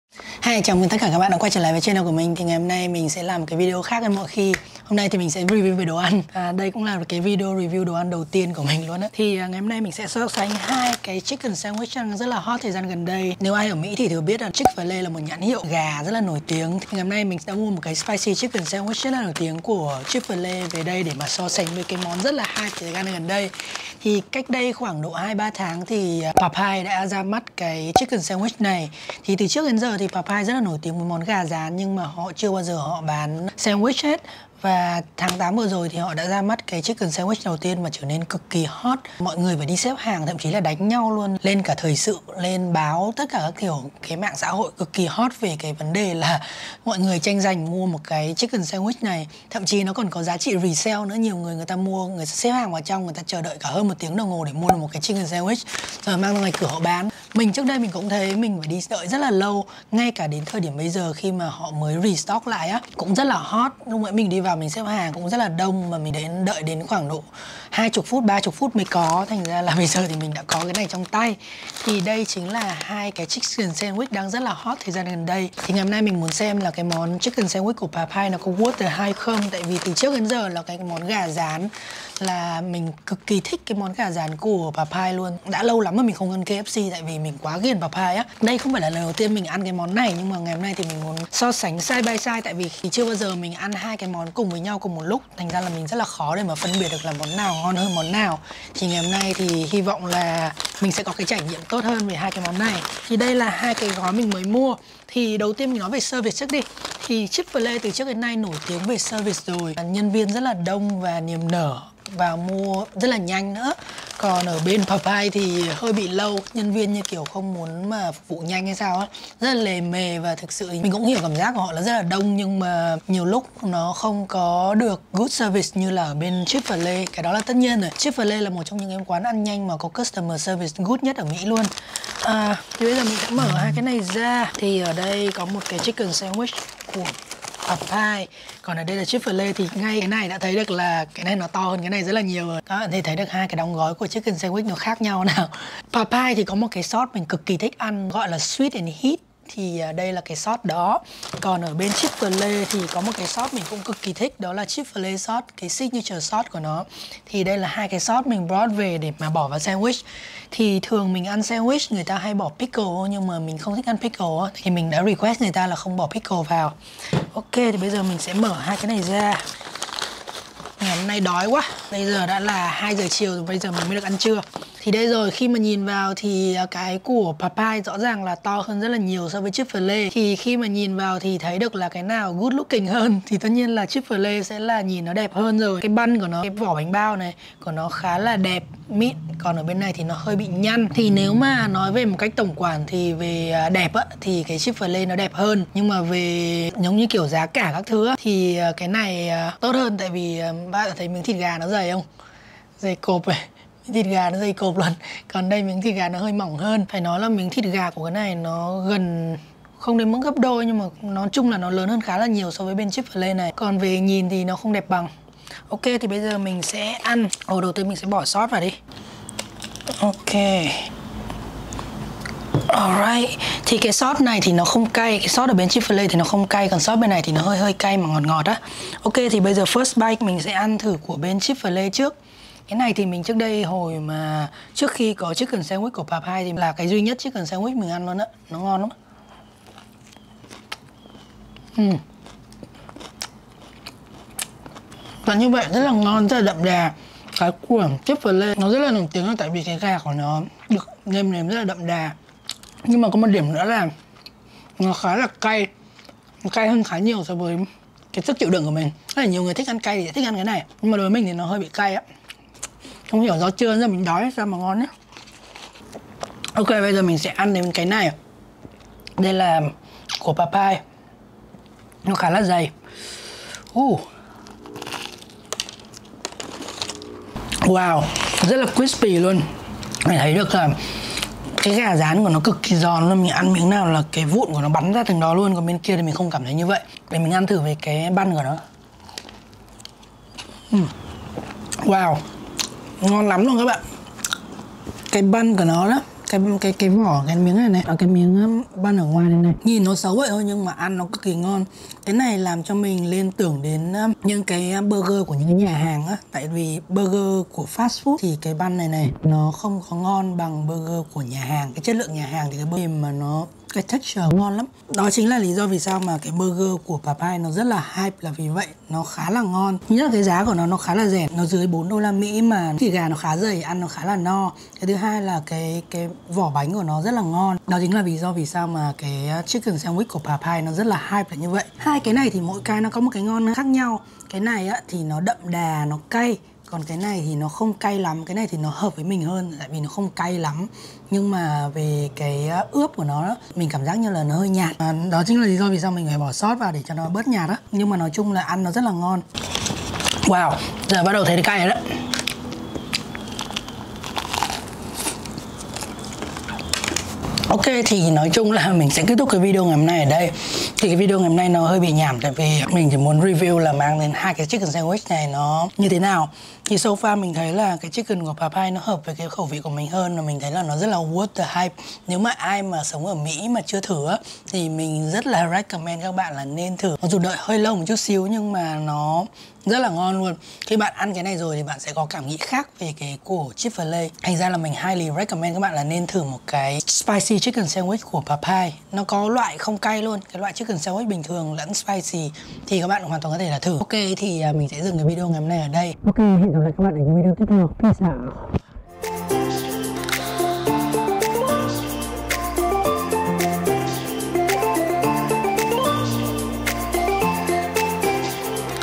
Xếp hàng 1 tiếng để ăn 1 cái Chicken Sandwiches - Is it worth the hype?
© Bản quyền thuộc về Hung Dinh
© Copyright by Hung Dinh ☞ Do not Reup